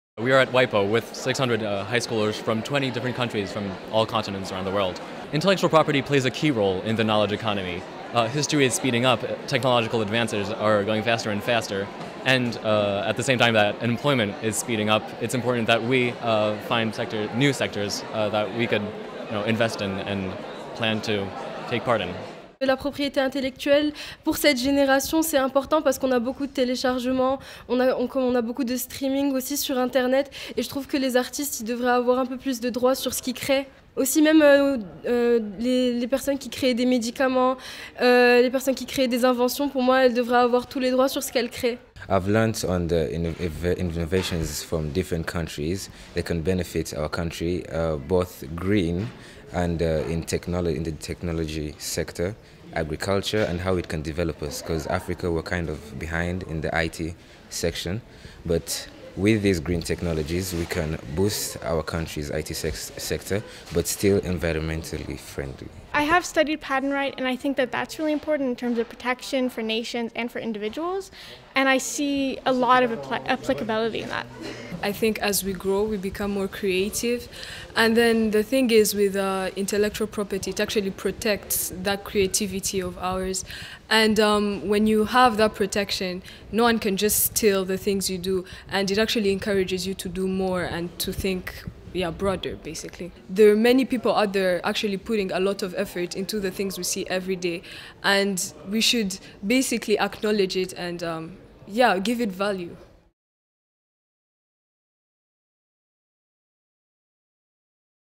Model UN Discusses IP at WIPO
Streaming, downloading, inventing: Some 600 teenagers from around the world discussed the future of IP at a "Model UN" meeting hosted at WIPO between January 11-13, 2017.